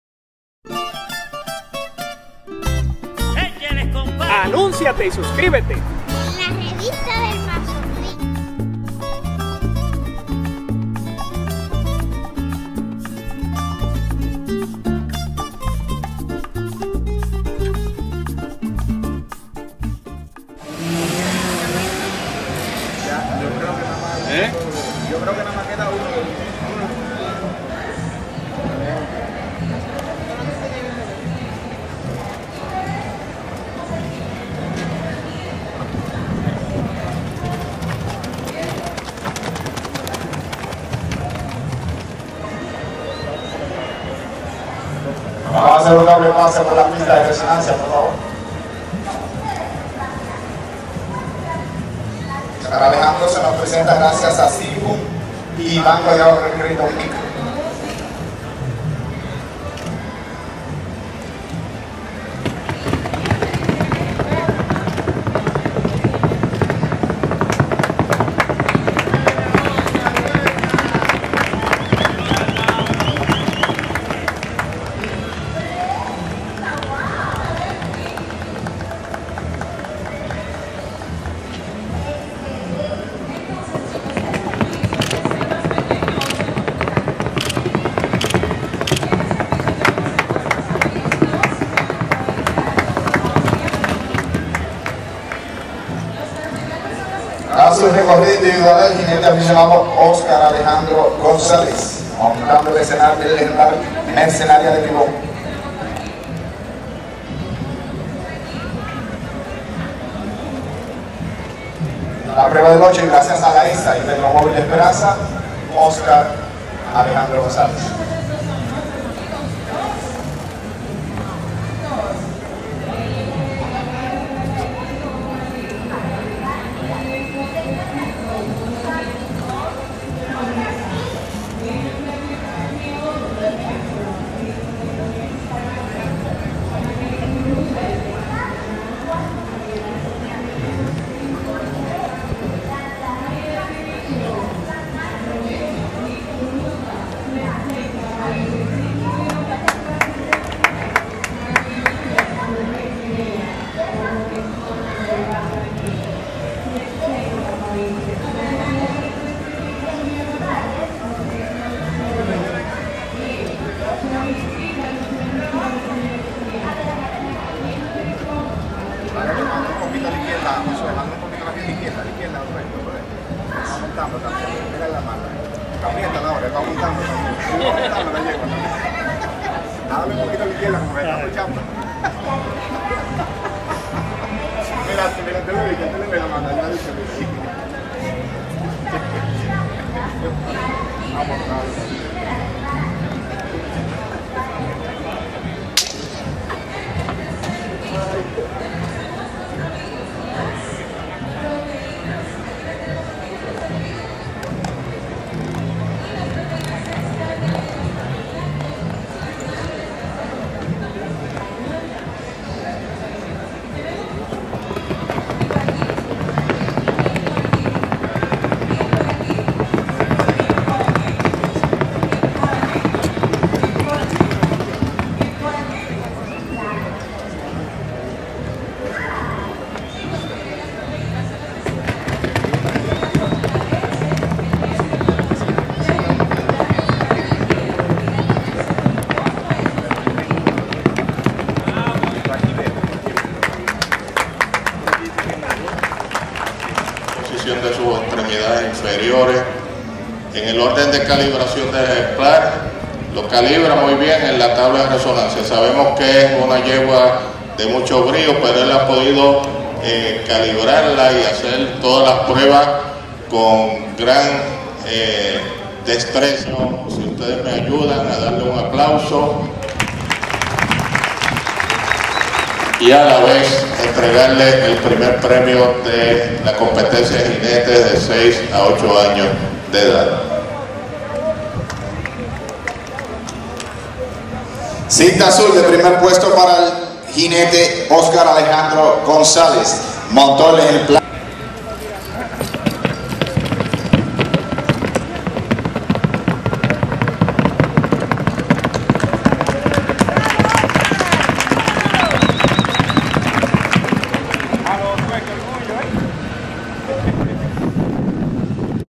Oscar Alejandro en Feria Agropecuaria Nacional 2012
Participacion de Oscar Alejandro Gonzalez en la Feria Agropecuaria Nacional 2012.
Comeptencia de Jinetes Infantiles de 6-8 años. Competencia Organizada por ADOPASO en La Ciudad Ganadera de Santo Domingo. Marzo 24, 2012.